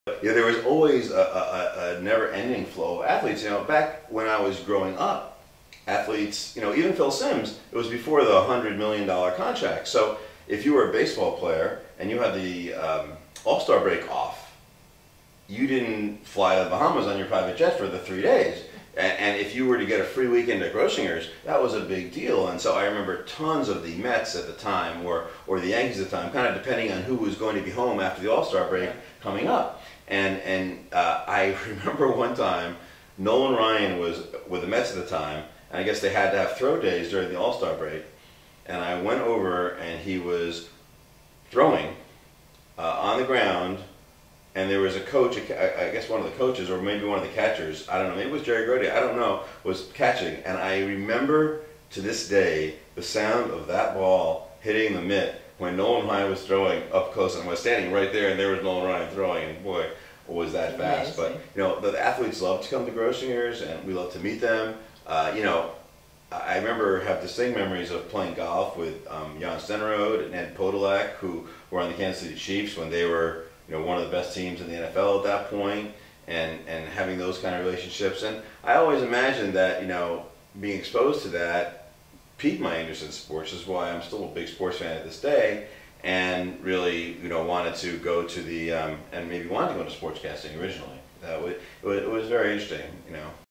Mitchell Etess with Elaine Grossinger
Elaine Grossinger Etess and her son Mitchell discuss the array of athletes that vacationed at Grossinger's Hotel in the Catskills.